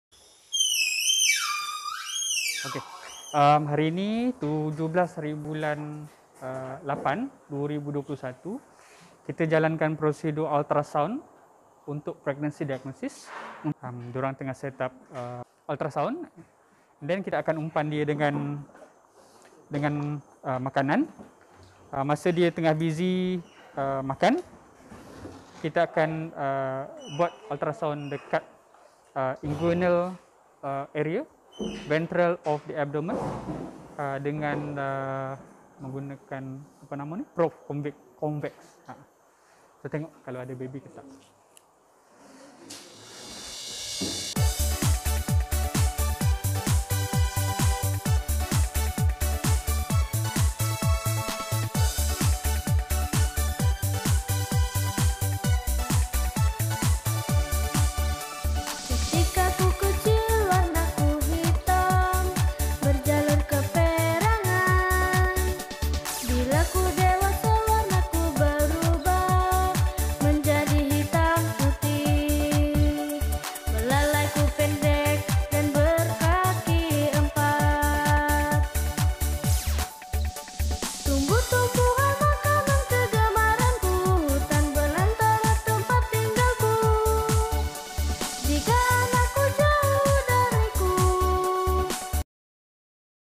#27 Malayan tapir pregnancy diagnosis using ultrasound
This female was diagnosed as pregnant by Dr Donny Y after it shows three readings of P4 hormone above 2.5ng/mL which indicate pregnancy. Mating was observed in March 2021, P4 hormone analysis done in may 2021. Expected to deliver at the end of april 2022.

Update: This tapir give birth on 25th April 2022